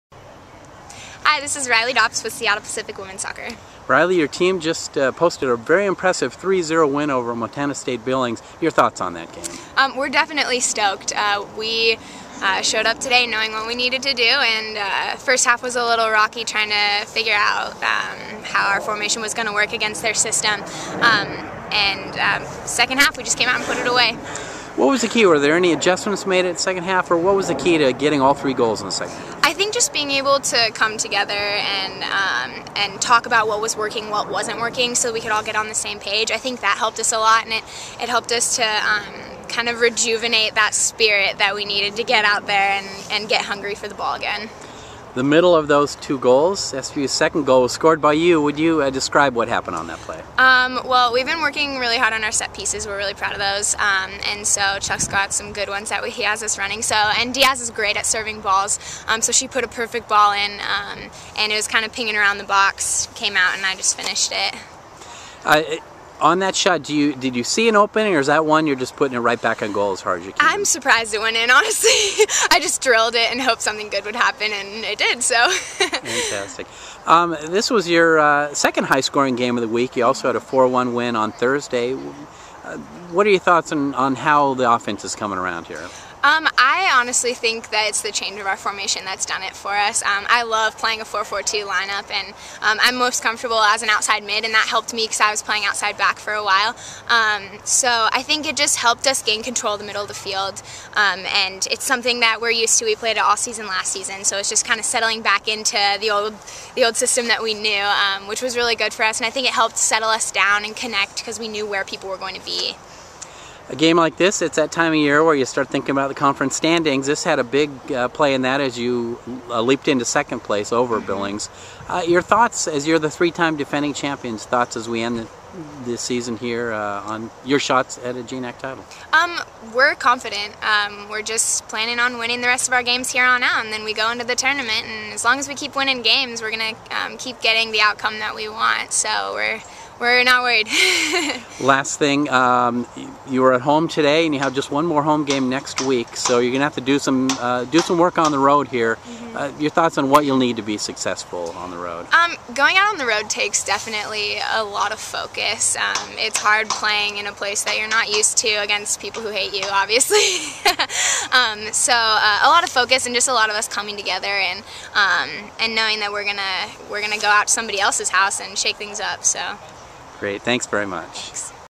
SPU SOCCER: Riley Dopps (Oct. 13, 2012)
Seattle Pacific sophomore Riley Dopps talks about scoring the team's second goal in a 3-0 women's soccer win over Montana State Billings.